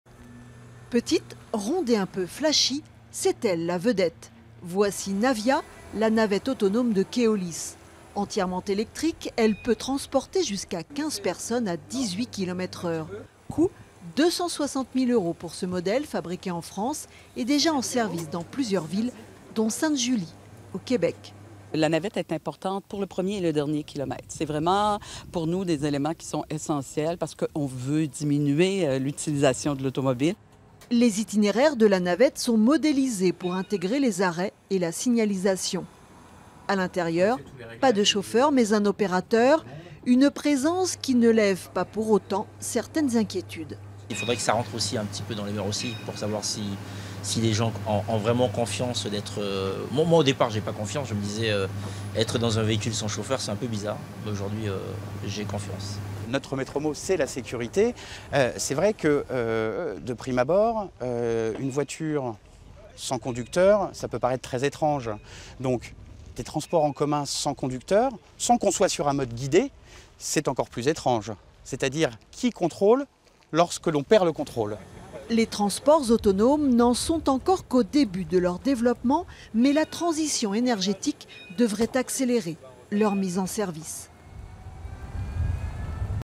Châteauroux : découverte de Navia, une nouvelle navette autonome
Faut-il rendre les transports publics gratuit ?

C’est le thème des rencontres internationales du transport qui se tiennent à Châteauroux, une ville où les bus sont gratuits depuis 2002.

Les participants ont testé une nouveauté : la navette autonome !

Reportage : Corinne Bian Rosa / Philippe Roy